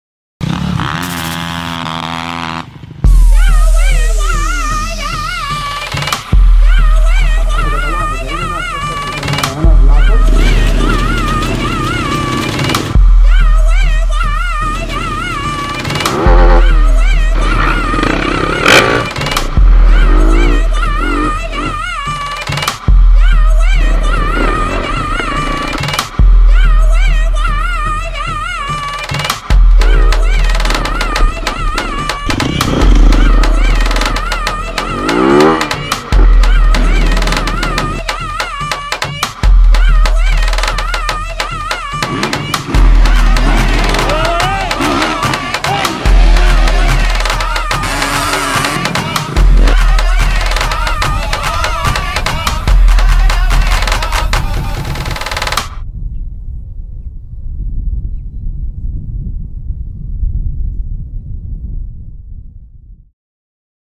Short Film "Enduro race" / Greece
1η Φιλική Εντουροβόλτα Κόνιτσα 7.9.2019

Εντουροβόλτα στα βουνά της Κόνιτσας, μέσα απο φαραγγια,στενα μονοπάτια,χωρια,ποτάμια κ.α .

Filming: Konstantinos Nikolopoulos, Eirini Siga
Editing: Konstantinos Nikolopoulos